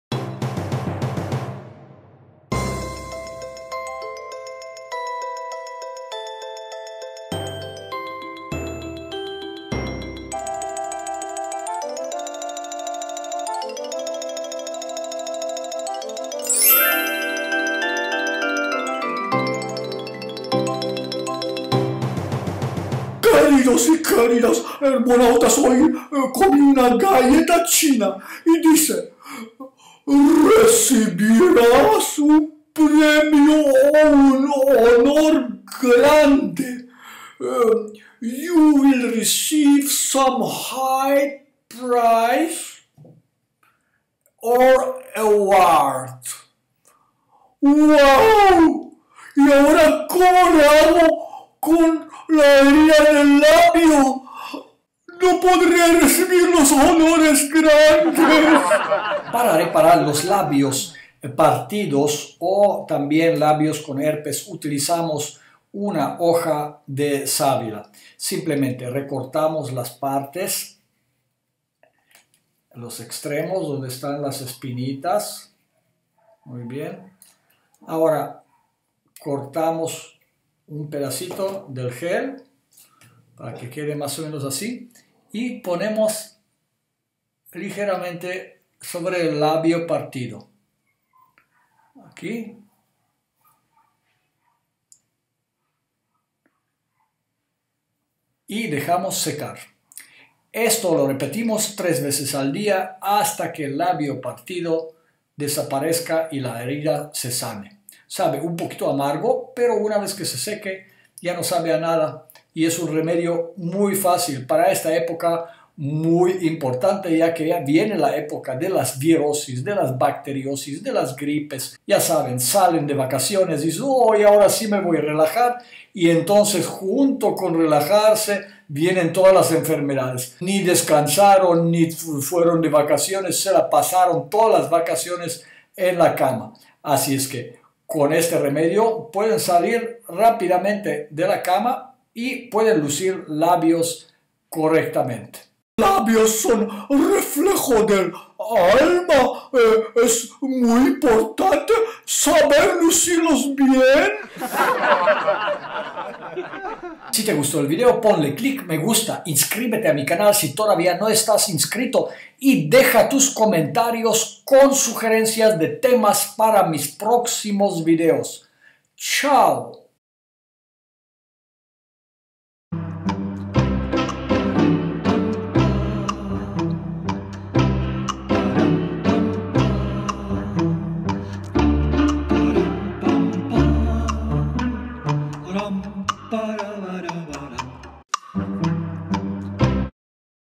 Los labios partidos y sangrantes
Remedio infalible para los labios partidos y sangrantes.
Dale manita arriba para mucho mas vídeos como este, comparte con tus amigos y déjame tus comentarios.
Hit little hand up to much more videos like this, share with friends and leave me your comments.

Categoría: Formación